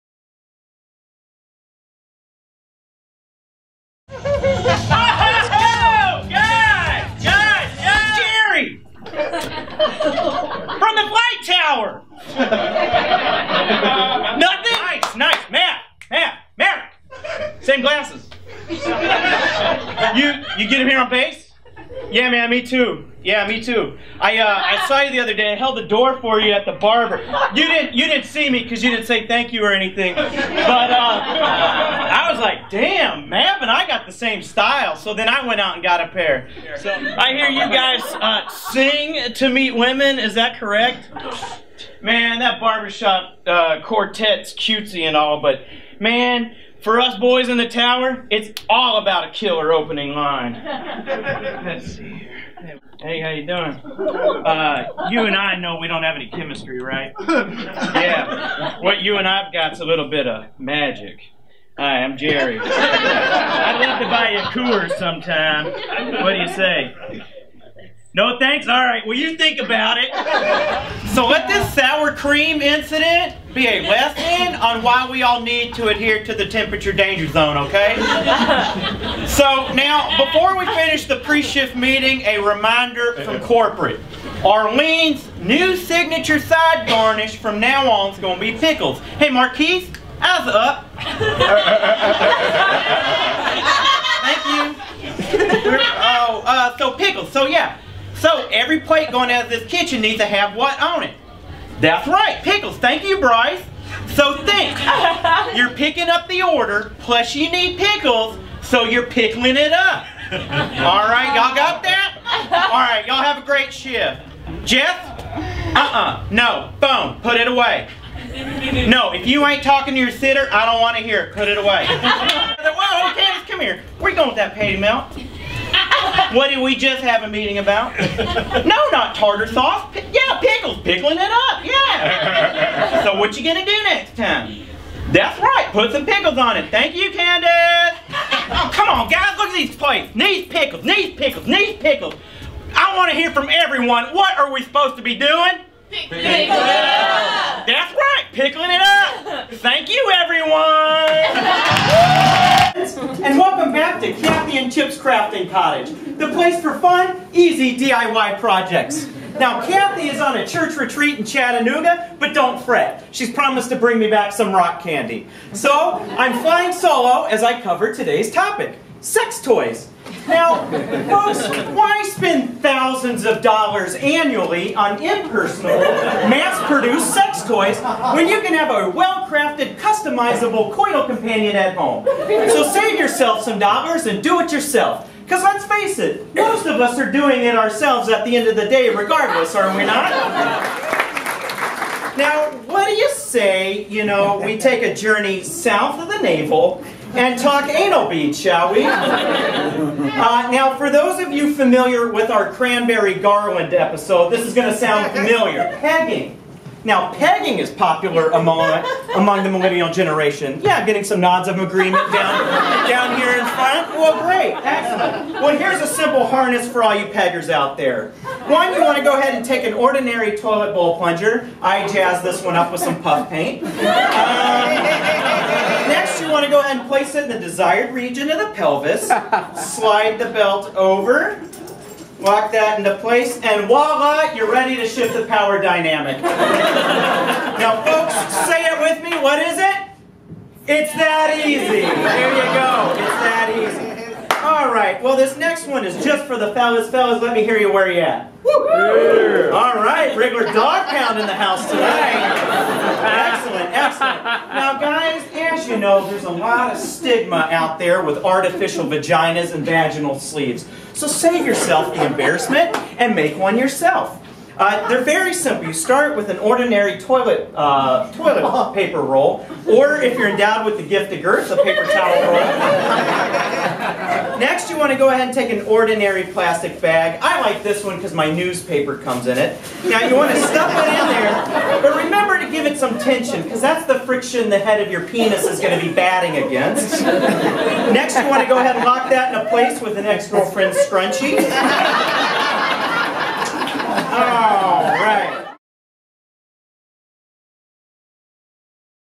Austin Rye Character Reel
3 original characters written and performed by Austin Rye. Recorded live at the UCB East and UCB Chelsea.